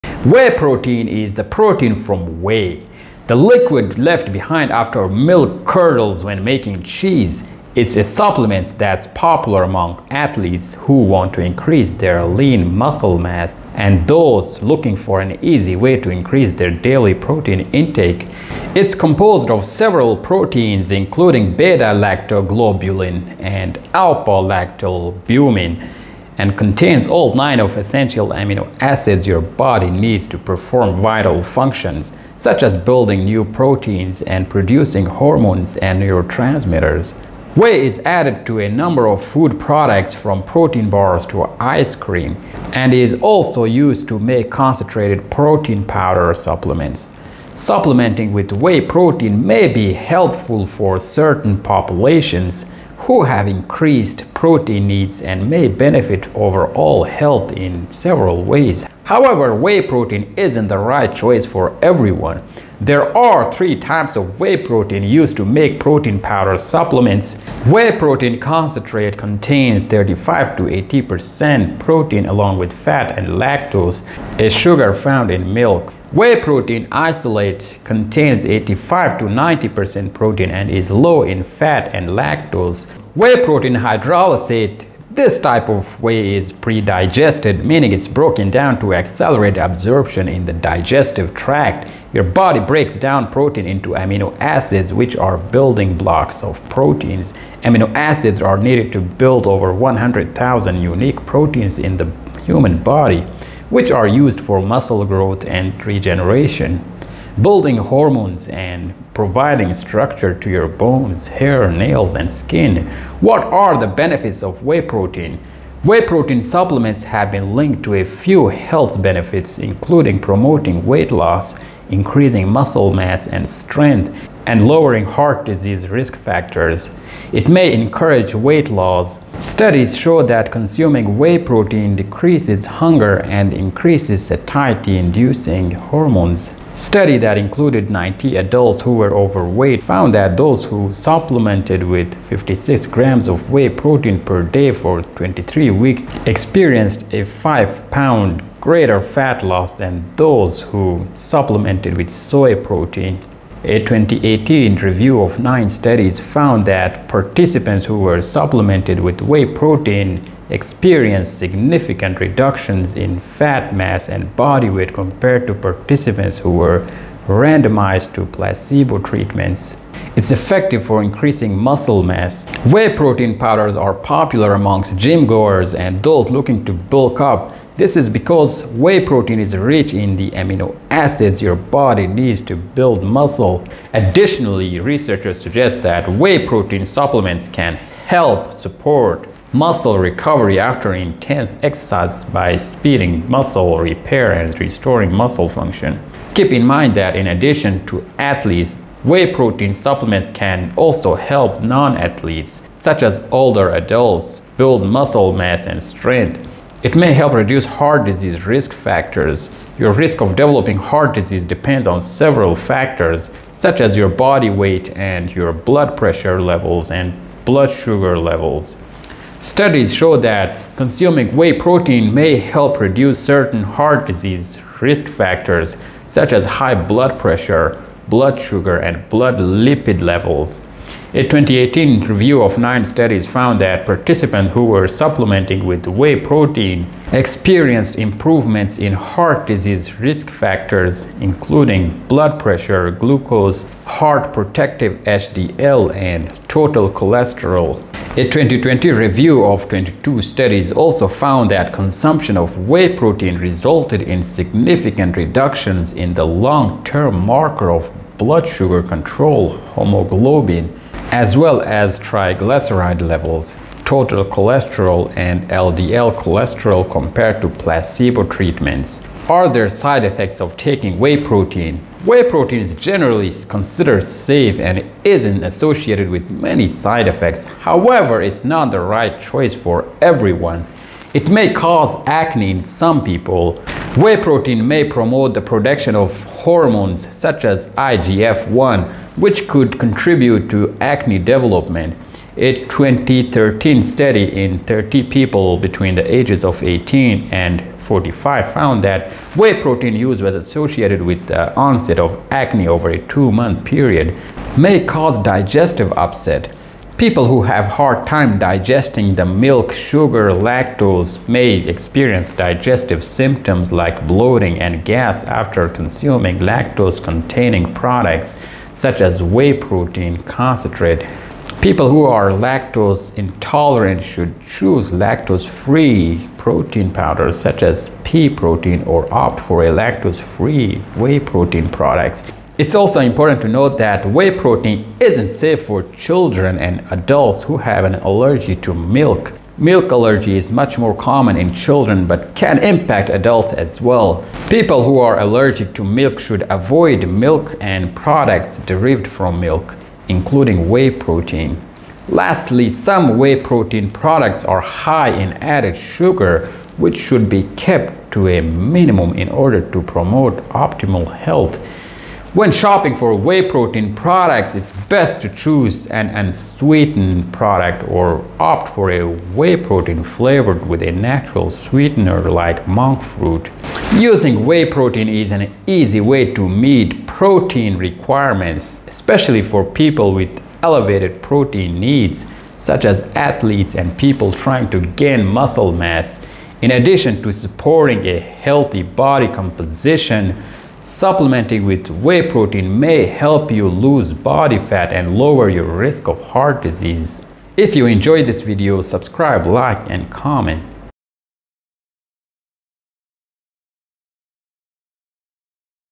What Is Whey Protein-And What Are Its Benefitrs?
Whey is added to a number of food products, from protein bars to ice cream, and is also used to make concentrated protein powder supplements. Supplementing with whey protein may be helpful for certain populations who have increased protein needs and may benefit overall health in several ways. However, whey protein isn’t the right choice for everyone.

What Are the Benefits of Whey Protein?
Whey protein supplements have been linked to a few health benefits, including promoting weight loss, increasing muscle mass and strength, and lowering heart disease risk factors.

It May Encourage Weight Loss
Whey protein may help reduce appetite, which can promote weight loss. Studies show that consuming whey protein decreases hunger and increases satiety-inducing hormones such as peptide YY (PYY) and glucagon-like peptide 1 (GLP-1).

A 2011 study that included 90 adults who were overweight or obese found that those who supplemented with 56 grams of whey protein per day for 23 weeks experienced a 5 pound (2.3kg) greater fat loss than those who supplemented with soy protein.

A 2018 review of nine studies found that participants who were supplemented with whey protein experienced significant reductions in fat mass and body weight compared to participants who were randomized to placebo treatments.

A small 2019 study that included 18 healthy adults found that participants who supplemented with hydrolyzed whey protein immediately before and after resistance training workouts three times per week for 12 weeks had greater increases of muscle circumference and strength compared to the placebo group.

Sports performance experts recommend consuming between 20-25 grams of leucine-rich protein, such as a whey protein shake, after resistance training in order to  maximize muscle growth. Leucine is an amino acid found in whey protein that plays an important role in protein synthesis and muscle repair.

Additionally, research suggests that whey protein supplements can help support muscle recovery after intense exercise by speeding muscle repair and restoring muscle function.

Keep in mind that, in addition to athletes, whey protein supplements can also help non-athletes, such as older adults, build muscle mass and strength.

It May Help Reduce Heart Disease Risk Factors
Your risk of developing heart disease depends on several factors, such as your body weight and your blood pressure levels and blood sugar levels.

Studies show that consuming whey protein may help reduce certain heart disease risk factors, such as high blood pressure, blood sugar, and blood lipid levels.

A 2018 review of nine studies found that participants who were supplementing with whey protein experienced improvements in heart disease risk factors including blood pressure, glucose, heart-protective HDL, and total cholesterol.

A 2020 review of 22 studies also found that consumption of whey protein resulted in significant reductions in the long-term marker of blood sugar control hemoglobin A1c (HbA1c) as well as triglyceride levels, total cholesterol, and LDL cholesterol compared to placebo treatments.

Are There Side Effects of Taking Whey Protein?
Whey protein is generally considered safe and isn’t associated with many side effects. However, it’s not the right choice for everyone.

It isn’t appropriate for vegans: Whey protein is derived from the milk of animals, such as cows and goats, so it’s not appropriate for people who exclude animal products from their diets.
It may cause acne in some people: Whey protein may promote the production of hormones such as IGF-1, which could contribute to acne development. A 2013 study in 30 people between the ages of 18 and 45 found that whey protein use was associated with the onset of acne over a 2-month period.

May cause digestive upset: People who have a hard time digesting the milk sugar lactose may experience digestive symptoms like bloating and gas after consuming lactose-containing products, such as whey protein concentrate. People who are lactose intolerant should choose lactose-free protein powders such as pea protein or opt for a lactose-free whey protein product.
It’s also important to note that whey protein isn’t safe for children and adults who have an allergy to milk. Milk allergy is much more common in children, but can impact adults as well.

People who are allergic to milk should avoid milk and products derived from milk, including whey protein.